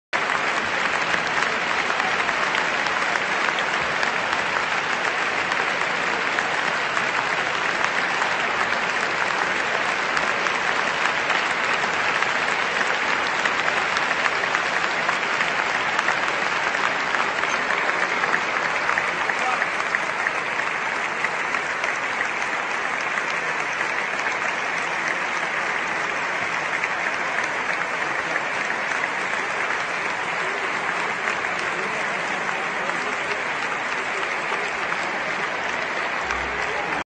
"Ciao Kobe": a Bologna l'applauso della Fossa dei Leoni per Bryant
Uno striscione e un minuto di applausi durante l'intervallo del match contro Varese. CosÃ¬ i tifosi della Fortitudo ricordano Kobe Bryant. "Ciao Kobe", scrivono gli ultrÃ  della Fossa dei Leoni in uno striscione (di LUCA BORTOLOTTI)